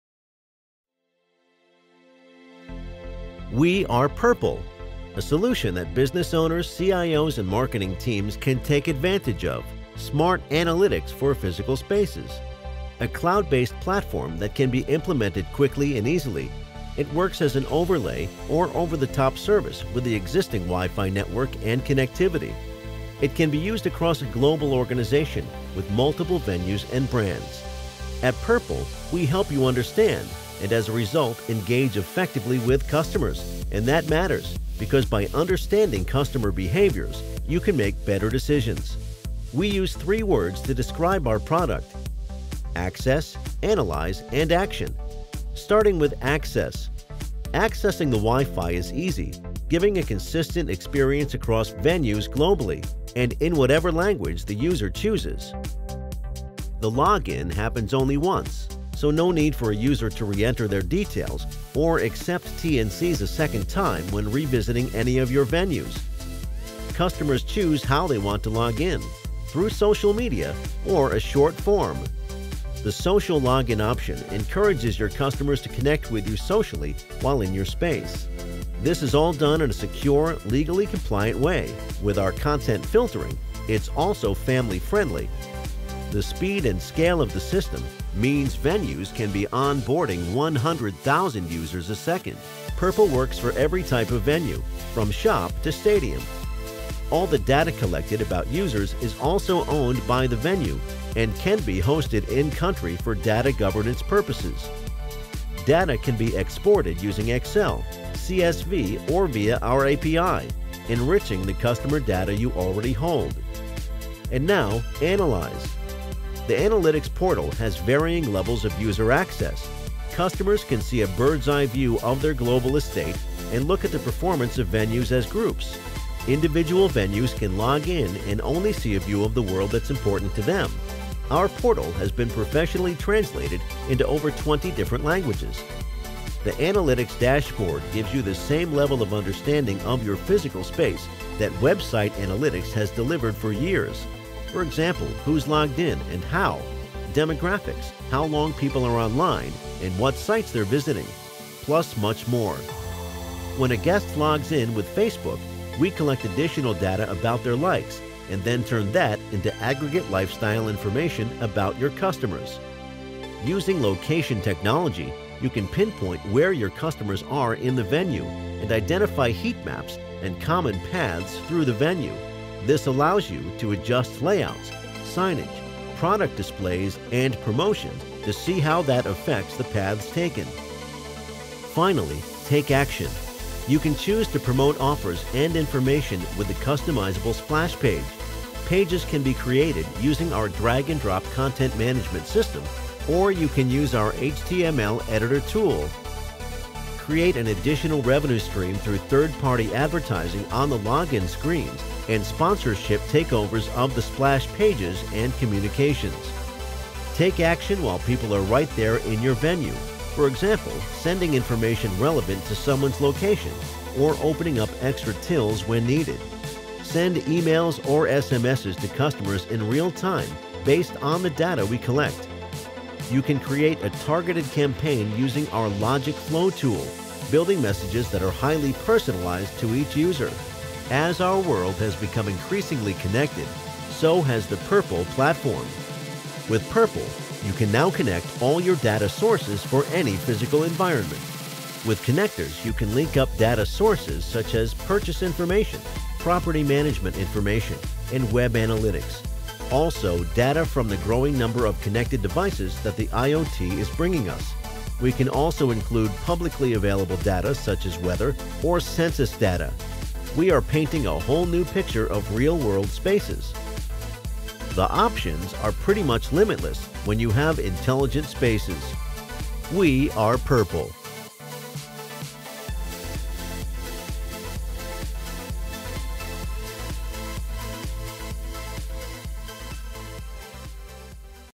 Purple Enterprise Product Video
Need a GDPR compliant way to engage with your customers and encourage repeat visits? This video offers an insight into social wifi with Purple AI. With options of Branded splash pages, targetted marketing messages content filtering custom page redirects and much more. Please email or call to see how this could help you connect with your customers and work for your business.